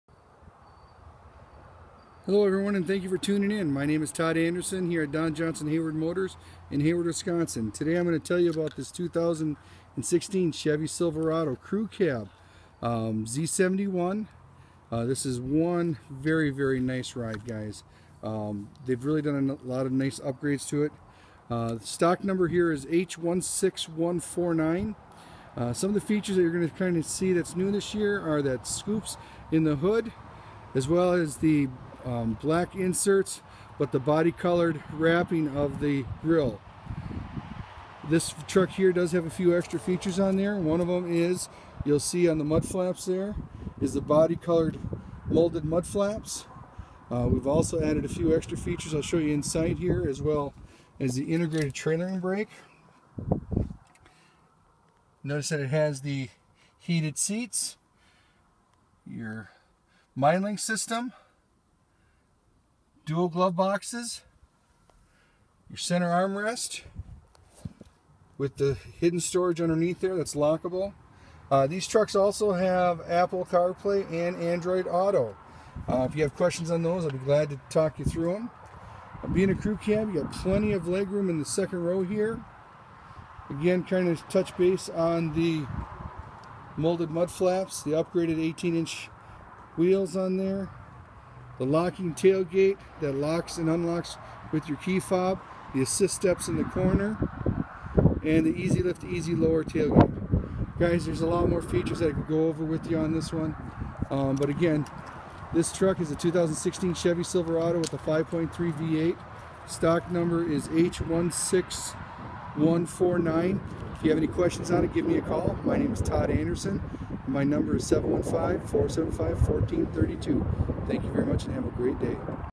2016 Chevrolet Silverado at Don Johnson Hayward Motors in Hayward, WI
Todd Anderson, Product Expert at Don Johnson Hayward Motors, in Hayward, WI, shares this 2016 Chevrolet Silverado Crew Cab . Stk# H16149